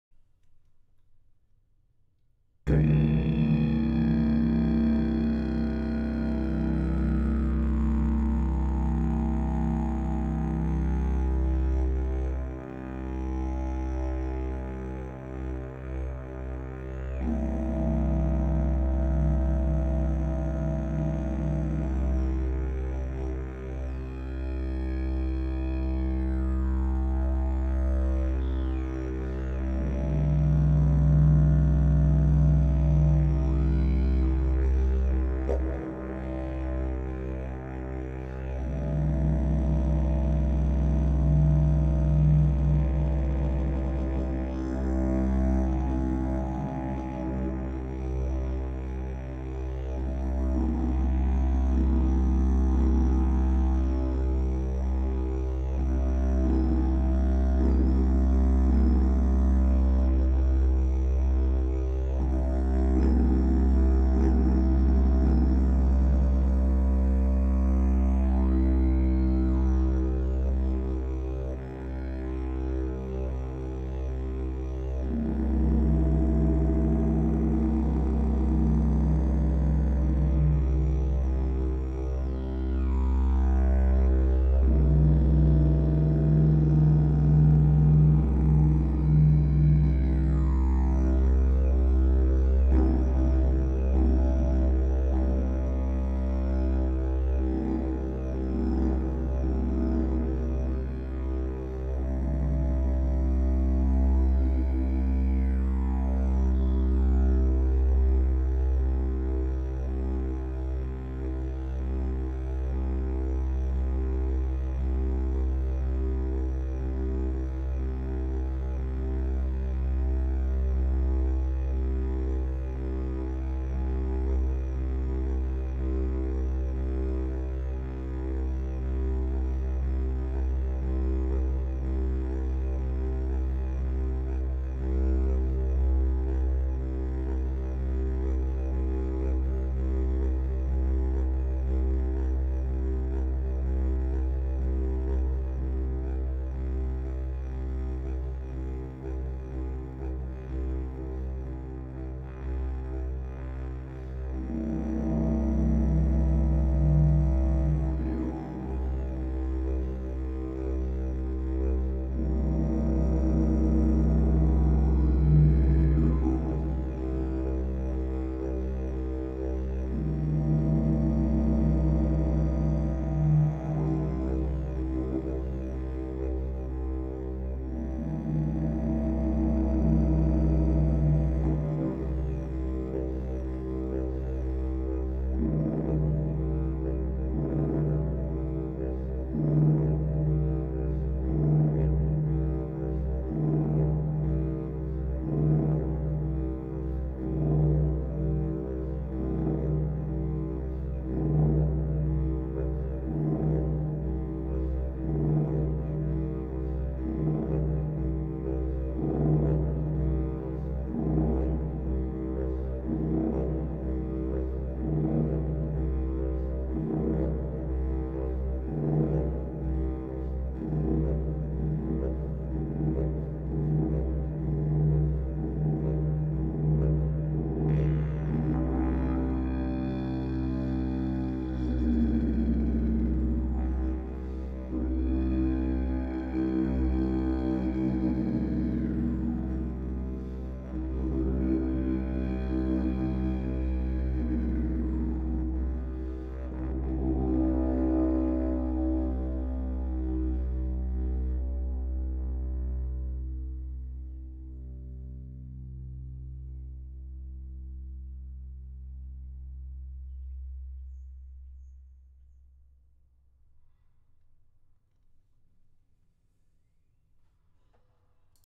Big Yucca Didge + Reverb
Hello, my friends! Wow, it has been forever since I've posted anything. I've been in school for 18 months, and... well... I suppose I just got shy about sharing my playing. There's so much more pressure when one suddenly gets noticed for certain videos. Anyway, I decided to go back to keeping it simple, so here's some droning, vocals, and a wee bit of rhythm, all processed through a bunch of reverb in realtime via my Universal Audio DAW. This will sound WAY better in headphones or on decent speakers.